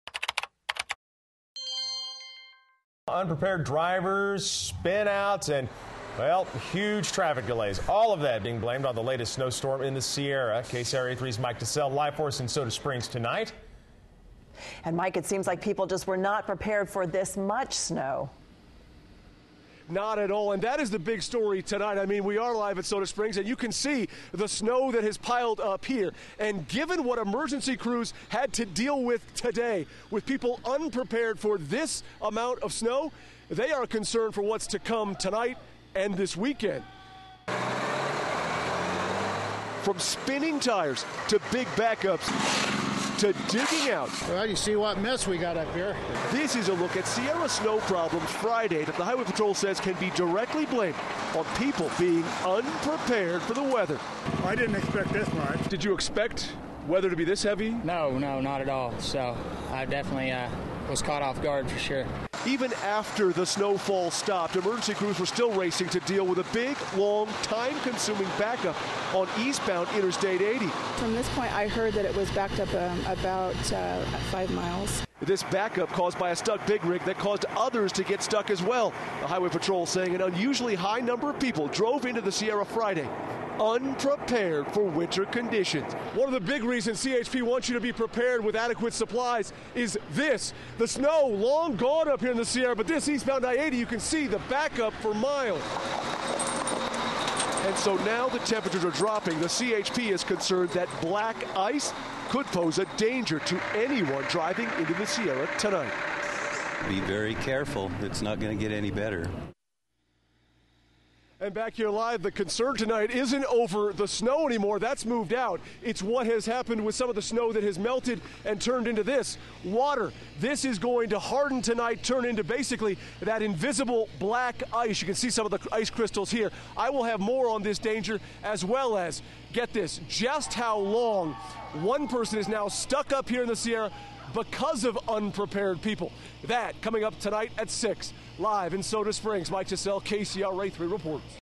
Not Prepared For Snow
With more snow coming emergency crews are worried travelers won't be prepared.